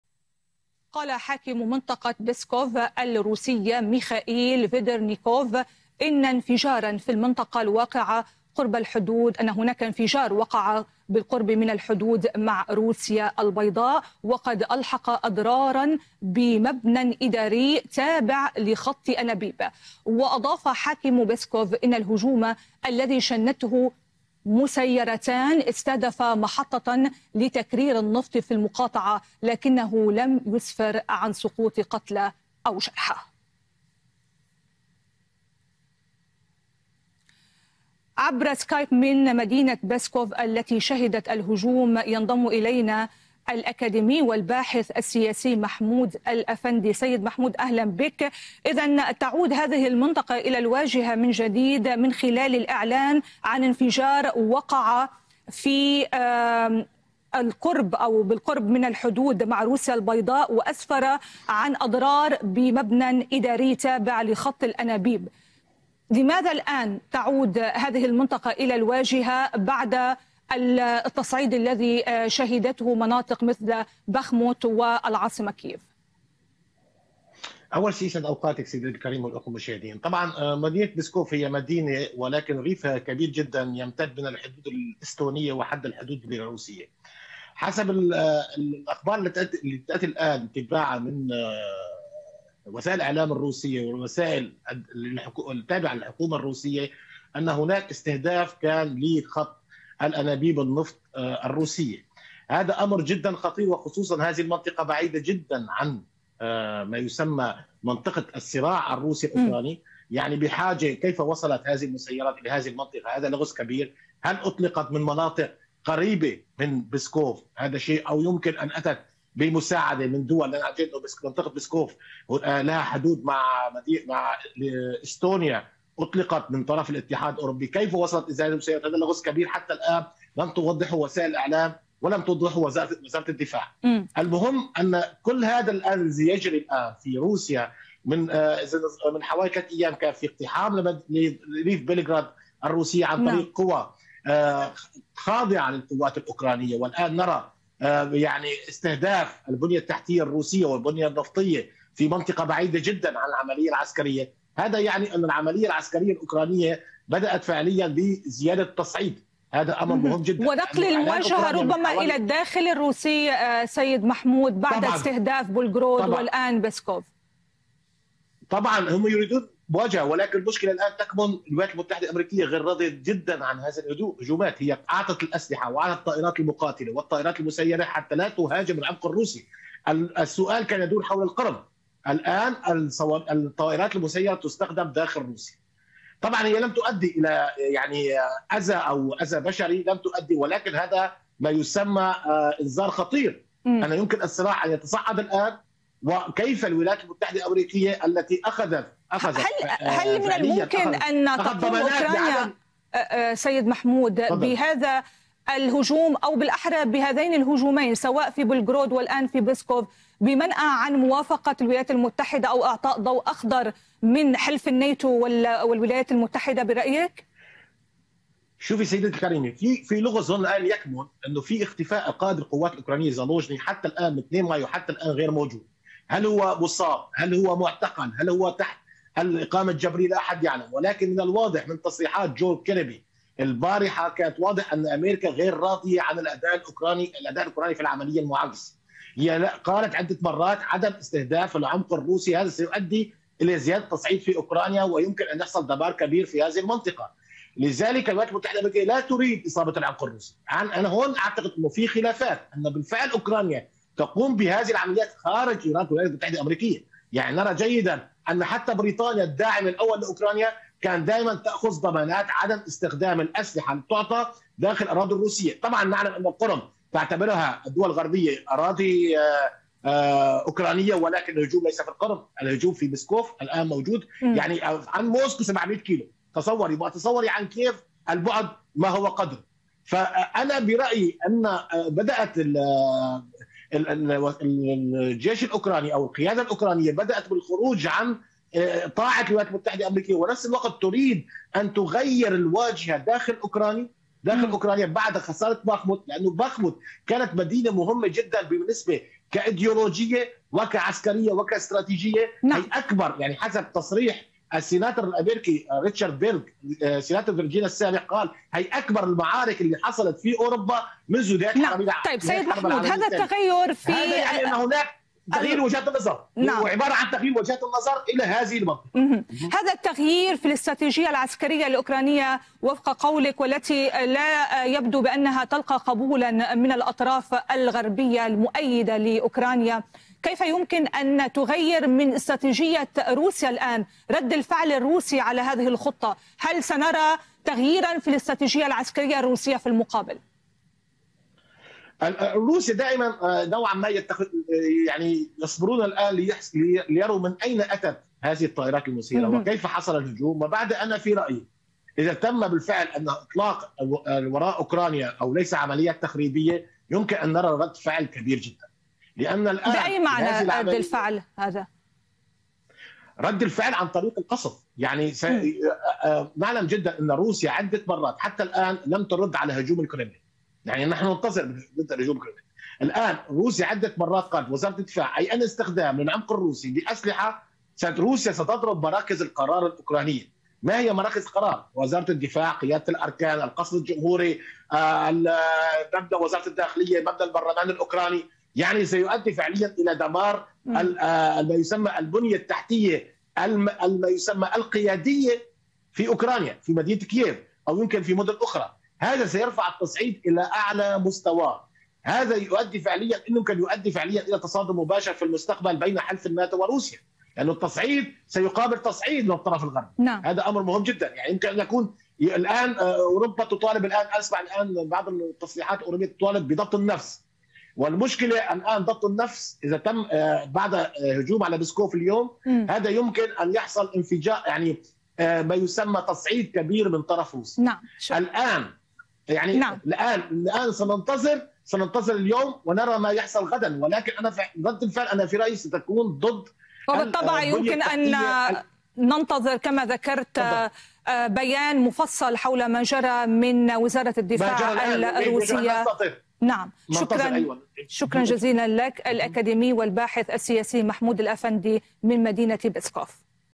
طائرات مسيرة تستهدف مدينة بسكوف الروسية .. هل بدأت أوكرانيا هجومها المضاد؟
أعلن حاكم منطقة بسكوف الروسية ميخائيل فيديرنيكوف على تطبيق تلغرام،  أن المدينة تعرضت لهجوم بطائرتين مسيرتين استهدف محطة لتكرير النفط في المنطقة.